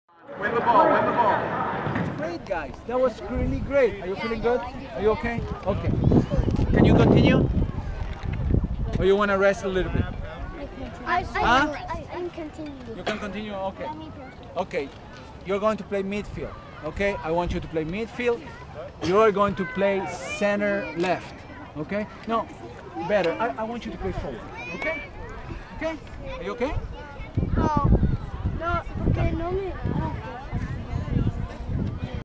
TOCATV: Toca Juniors U10 Boys Terrapins (before 2nd half starts)
Champions Classic vs Toca Juniors U10 Boys Terrapins